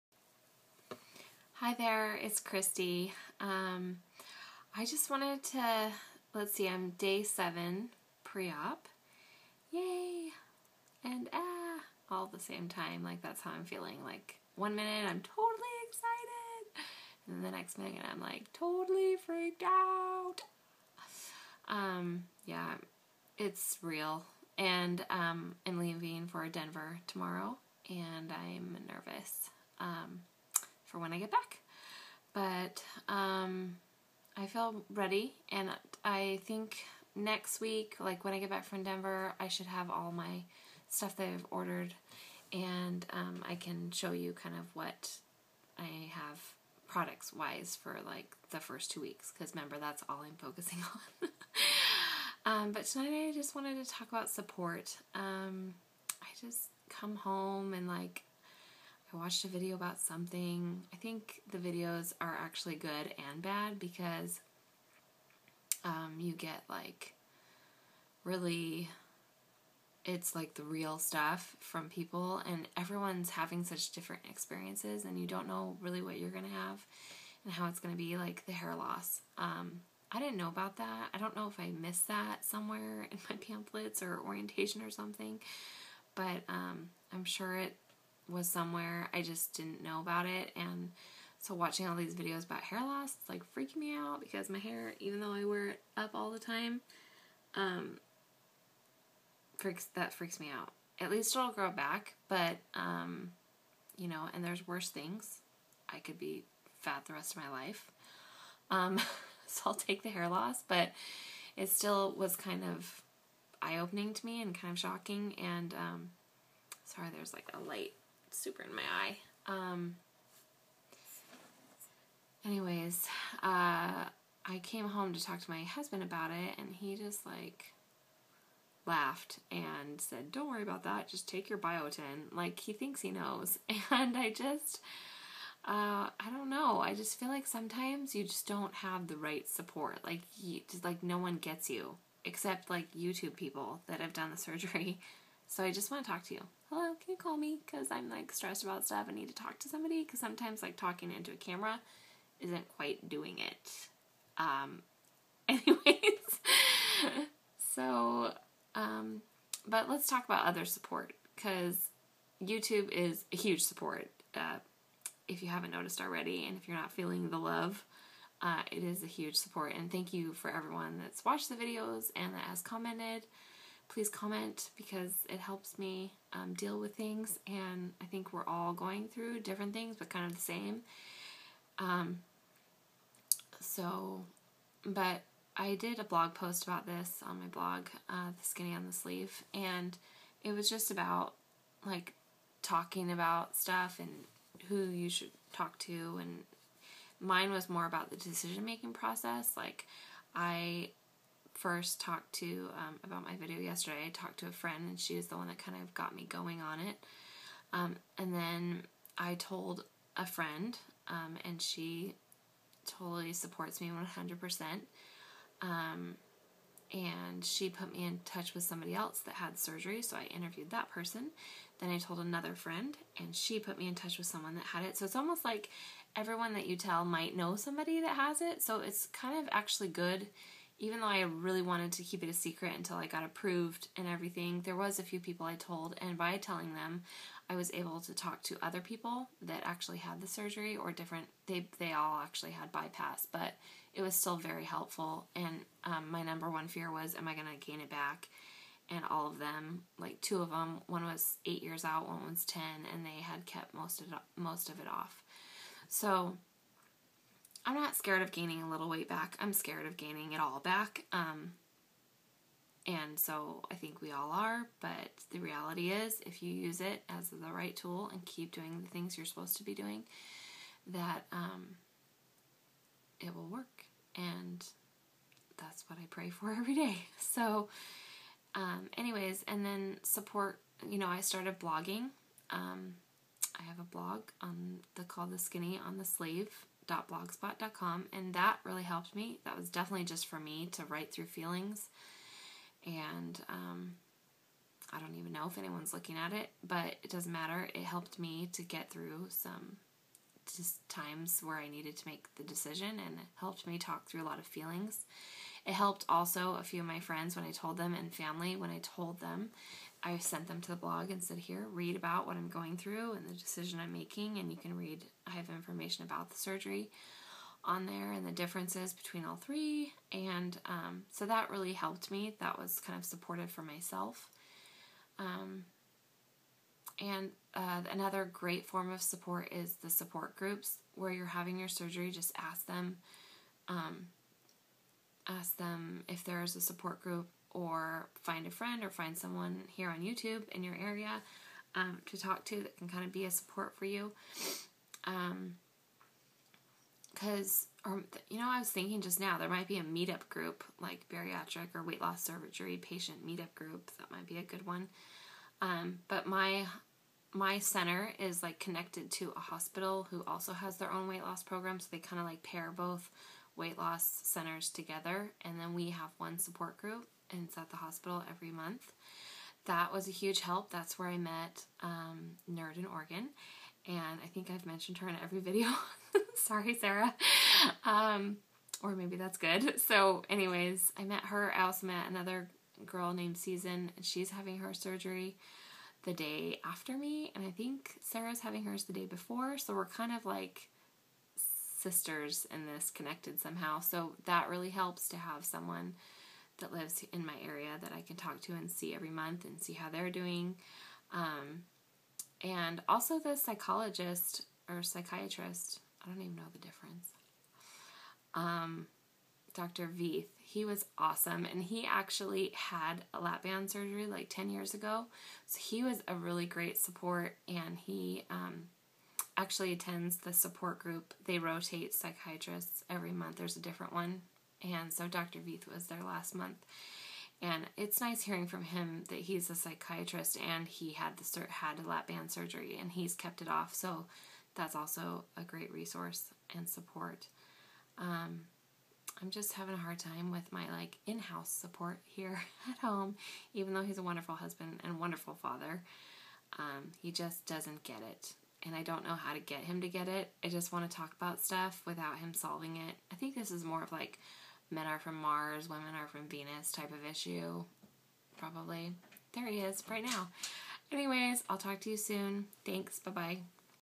VSG pre-op day 7: Support
My life and journey before and after Vertical Sleeve Gastrectomy (VSG) surgery.
This video goes into different forms of support you can find.